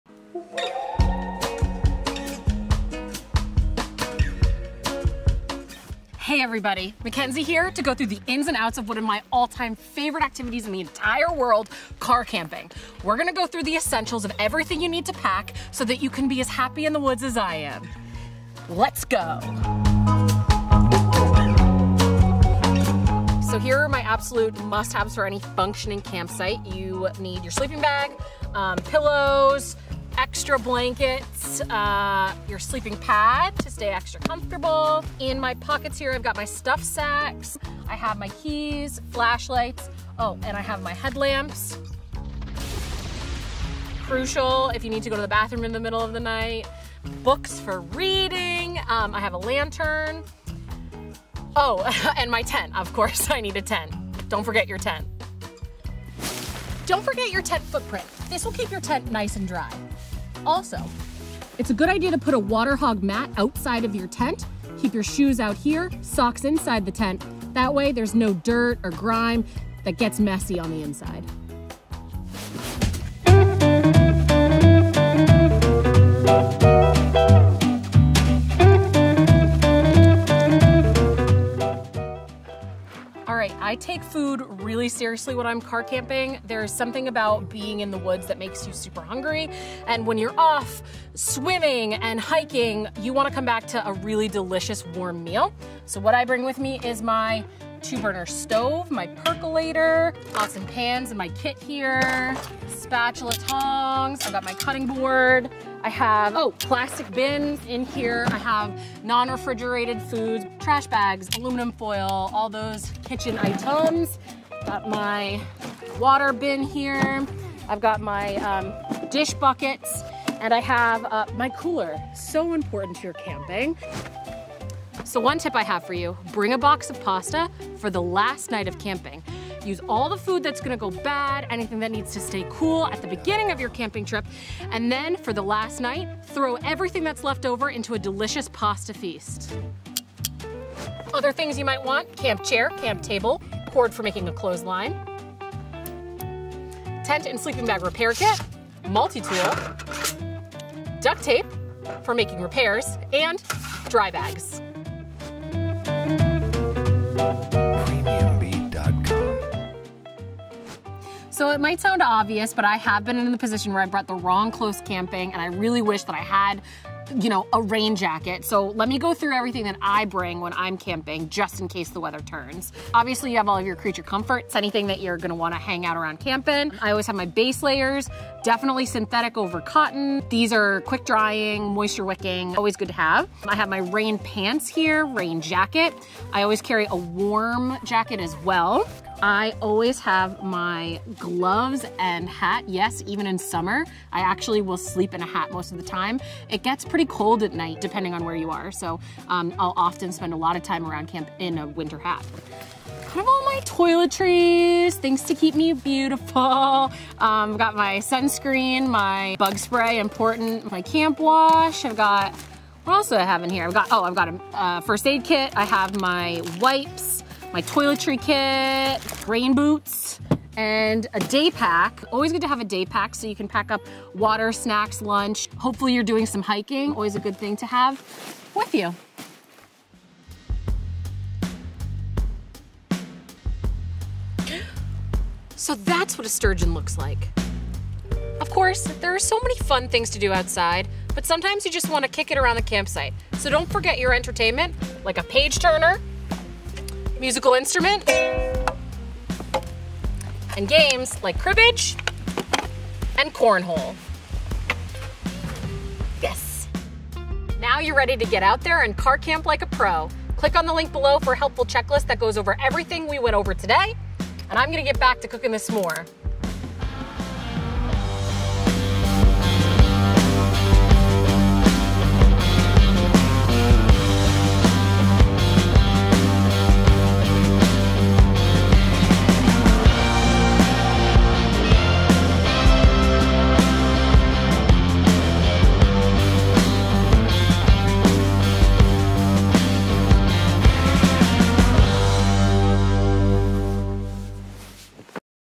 What to Pack for Car Camping
Making sure you bring the right gear on a car camping trip can be challenging. Mackenzie makes it fun and easy as she walks you through the gear you need for car camping.

For more expert advice on camping, stop by your local L.L.Bean or visit the Get Outside Guide

Chapters
0:00 Intro
0:21 The Basics
1:14 The Camp Kitchen
2:04 Cooking Tip
2:21 Camp Furniture & Miscellaneous Camp Gear
2:35 Clothing & Personal Gear
3:50 Fun & Games
4:17 Camping Checklist